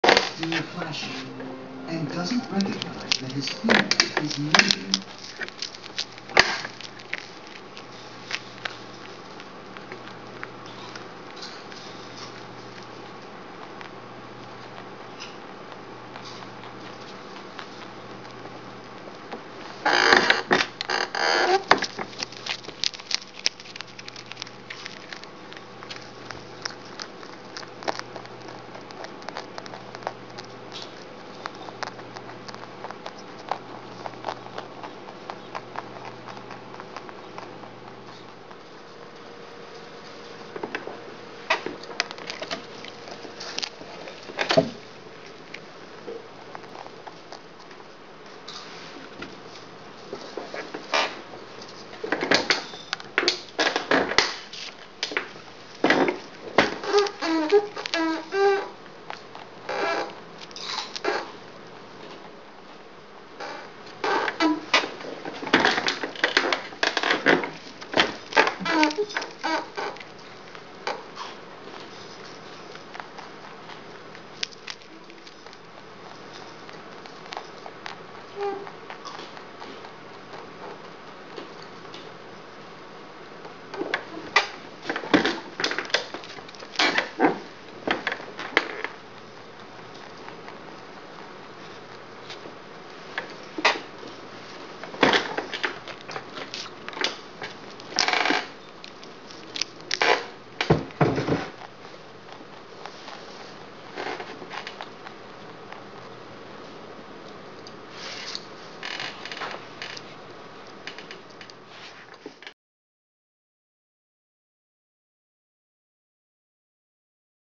Hector Likes Popcorn
Hector, our mini-lop rabbit, has trained us to give him things, like head rubs, or food, by nudging us on the feet.